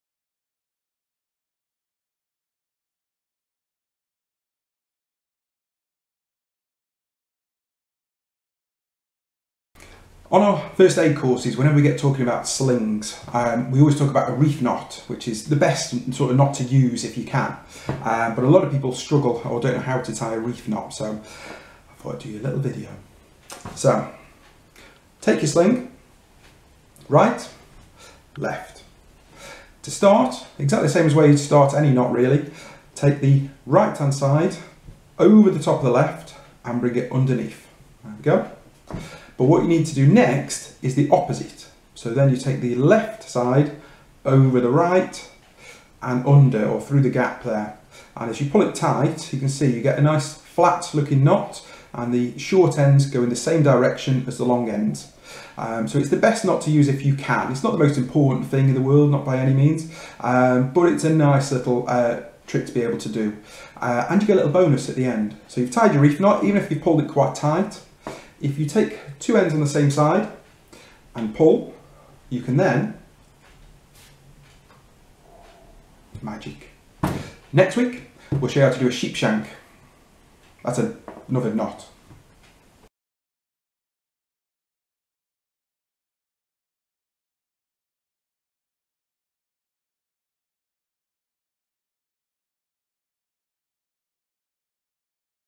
MIE How to tie a Reef Knot
We always discuss reef knots on first aid courses when practicing slings. A lot of people don't know how to tie one. so I made them a video!